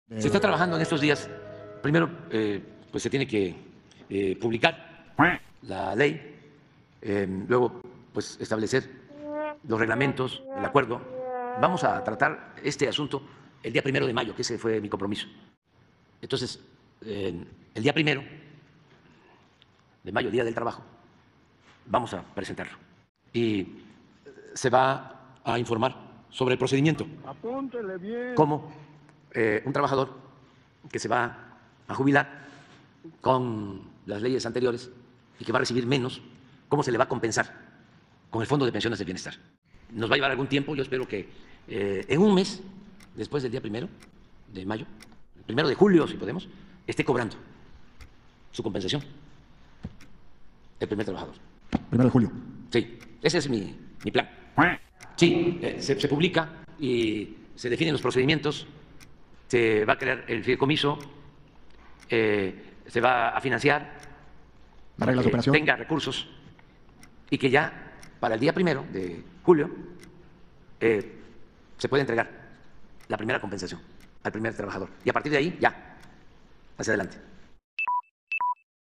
El 1 de mayo dará a conocer cómo funcionará el Fondo de Pensiones para el Bienestar 🦆
El 1 de mayo dará a conocer cómo funcionará el Fondo de Pensiones para el Bienestar 🦆, para que el 1 de julio, “el primer trabajador empiece a cobrar su compensación” 🦆